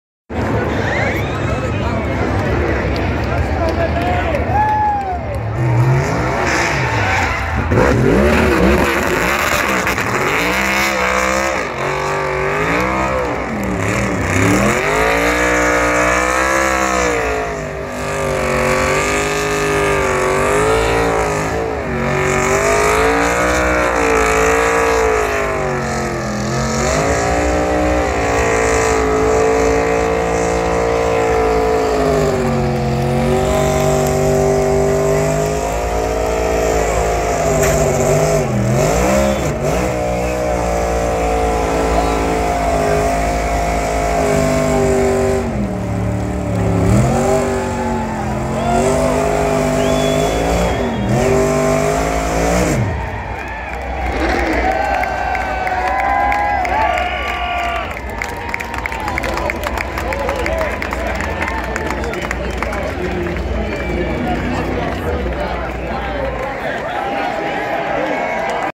Tyler Reddick Kansas Post Race Burnout
Tyler Reddick celebrates win at Kansas Speedway!